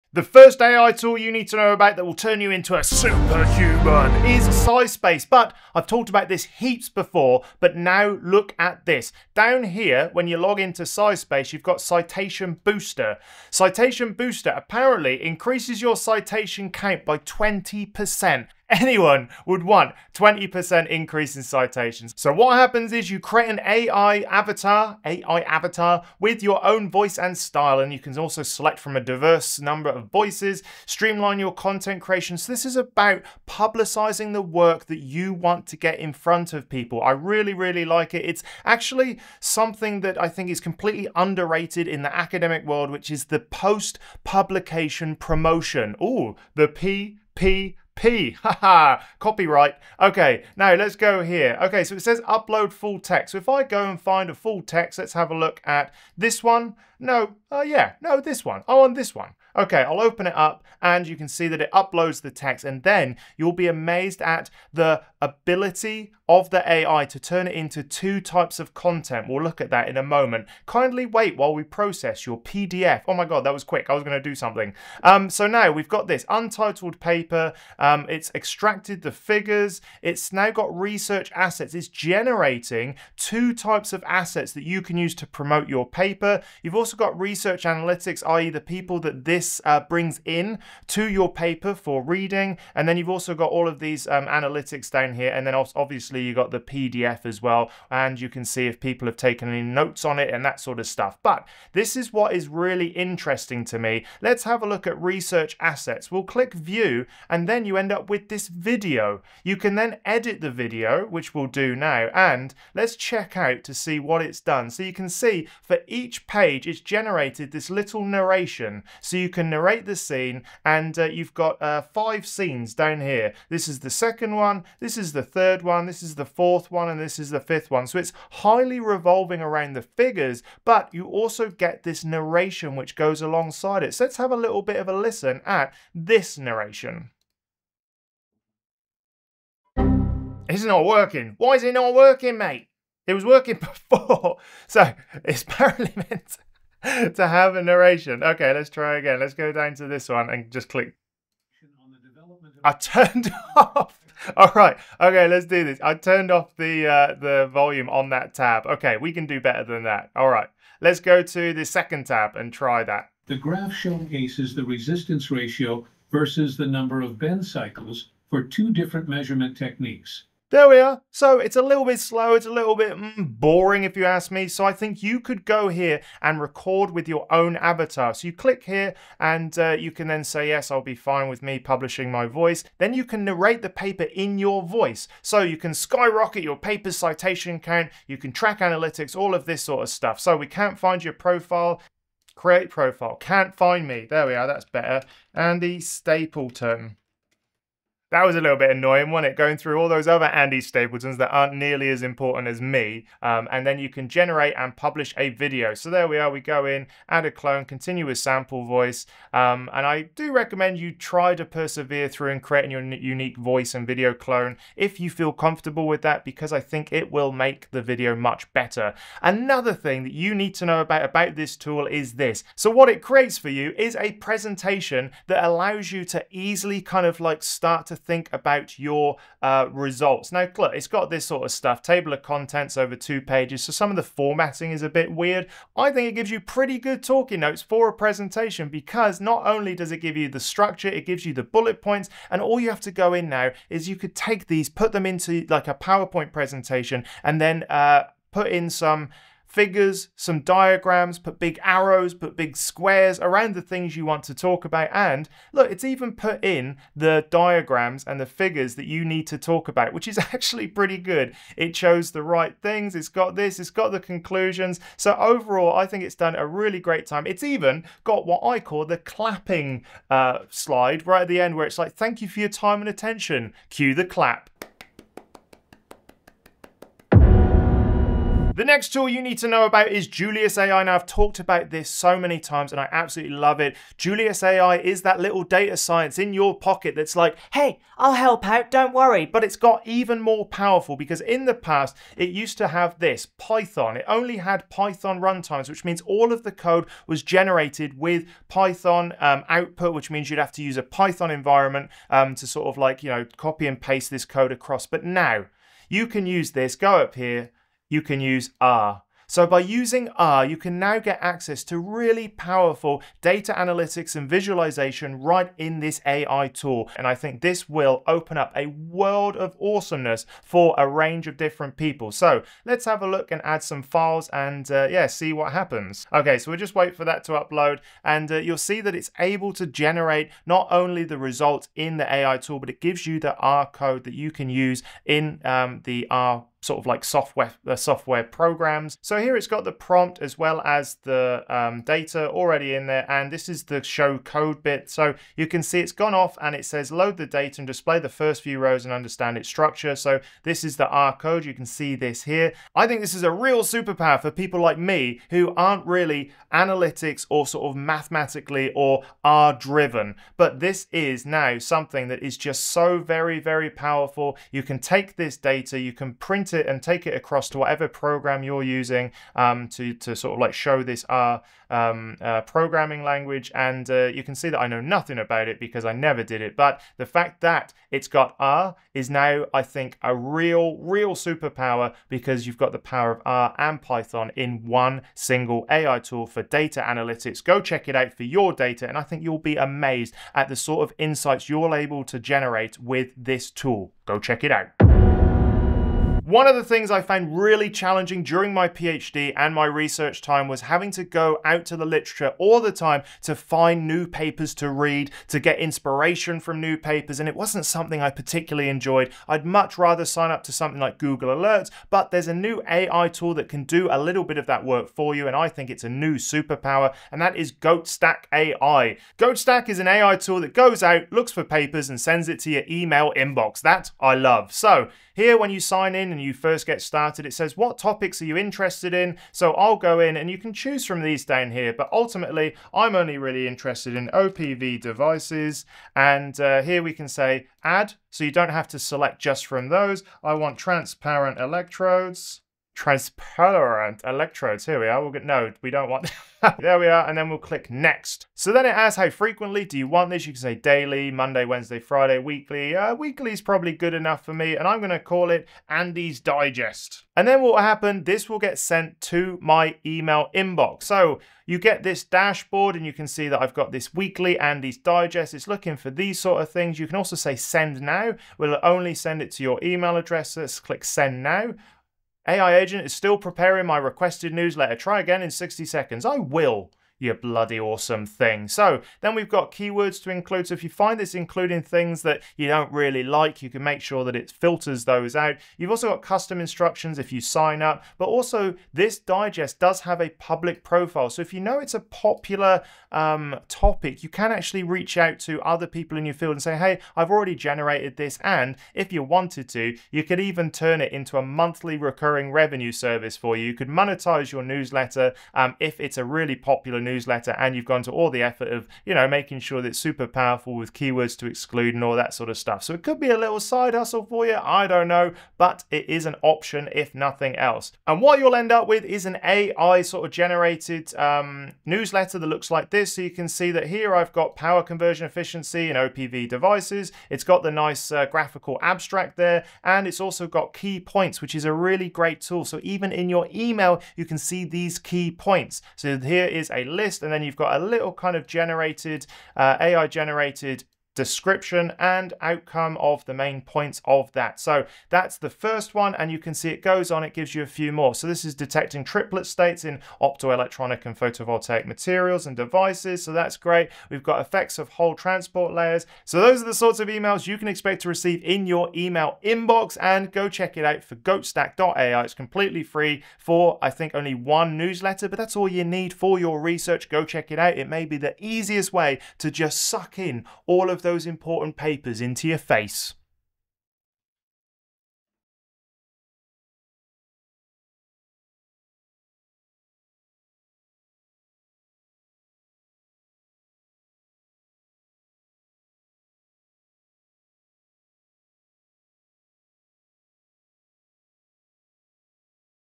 Become a Superhuman Researcher with These AI tools
In this video, I explore various AI tools that can significantly enhance the process of conducting and promoting academic research. As I walk through the features of these tools, I emphasize how they can make a researcher's life easier by automating tasks and increasing the visibility of published work. AI tools for research are becoming increasingly sophisticated, offering capabilities that go beyond simple automation. These tools can generate content, analyze data, and even assist in post-publication promotion, which is often overlooked but is crucial for increasing a research paper's impact.

One tool highlighted in the video is a platform that uses AI to boost your citation count by up to 20%. While some might question the need for such a boost, it's undeniable that increased citations can enhance a researcher's profile and influence in their field. The platform offers features like creating an AI avatar with your own voice and style, which can be used to narrate your research findings, making your work more engaging and accessible to a broader audience.

Another important aspect discussed is the ability to generate research assets automatically from uploaded papers. This includes creating videos that highlight key figures and narrate important points, which can be particularly useful for sharing research on social media or in presentations. This capability of AI research tools not only saves time but also ensures that the essence of the research is communicated effectively.

The video also touches on the integration of advanced programming languages like Python and R into these AI tools, making it easier for researchers to perform data analysis and visualization. This feature is especially valuable for those who might not be experts in coding but need to analyze complex datasets. By offering these functionalities, AI for research becomes a powerful ally in the academic process, helping researchers to focus more on their findings and less on the technical aspects.

▼ ▽ TIMESTAMPS
00:00 SciSpace
01:32 AI Avatar
03:23 Creating Avatar
03:42 AI Presentation
04:38 Julius AI
06:36 Goat Stack